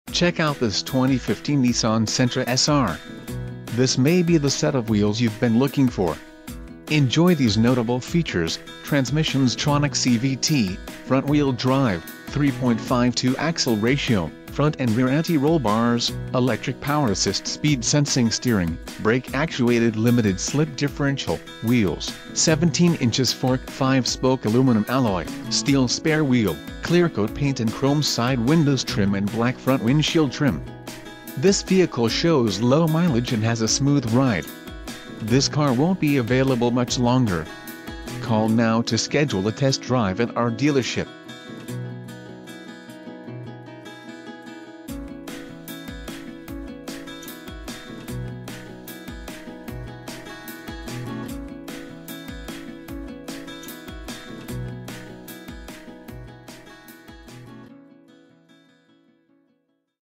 2015 Nissan Sentra SR - Kelly Car - Emmaus, PA 18049
2015 Nissan Sentra SR Condition: New Mileage: 4 Transmission: CVT with Xtronic Exterior Color: White Interior Color: Charcoal Doors: 4 Stock: N52111 Cylinders: 4